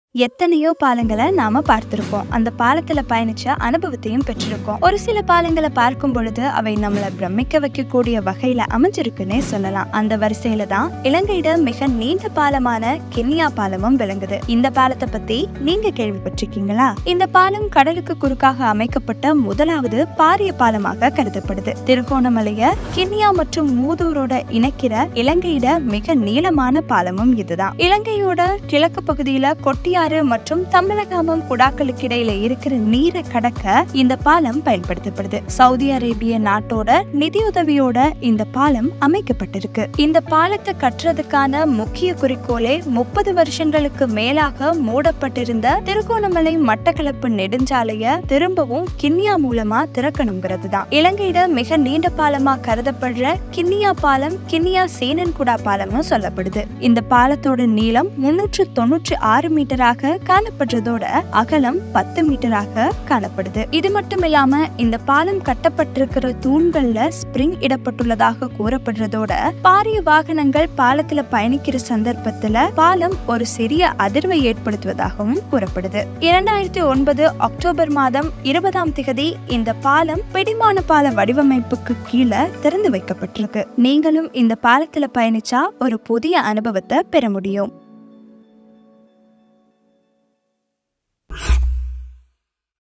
கடலுக்கு குறுக்கேசெல்லும் இலங்கையின் மிக நீண்ட கிண்ணியா பாலம் | Nadi
எத்தனையோ பாலங்களை நாம பார்த்துள்ளோம். அப்பாலத்தில் பயணித்து அனுபவத்தையும் பெற்றுள்ளோம்.

ஒரு சில பாலங்களை பார்க்கும் பொழுது அவை நம்மை பிரமிக்க வைக்கிற வகையில அமைந்திருக்கின்றன.
இந்த மாதிரி ஒரு பாலத்துல பயணித்து சூப்பரான ஒரு அனுபவத்தை நீங்களும் பெறலாம்.

வாழ்க்கை வாழ்வதற்கே!